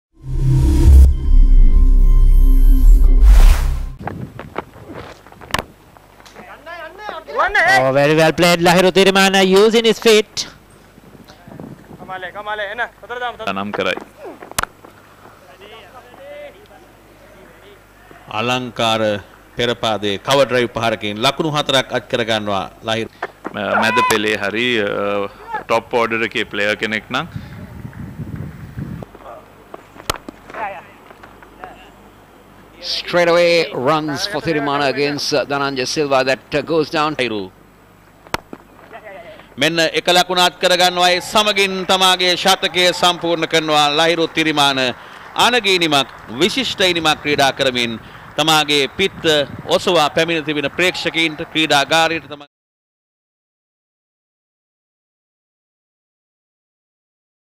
Lahiru Thirimanne - 125* in 258 balls
Team Colombo middle order batsman Lahiru Thirimanne scored 125* off 258 balls in their SLC Super Fours Provincial Tournament 2018 match against Team Galle in Hambanthota.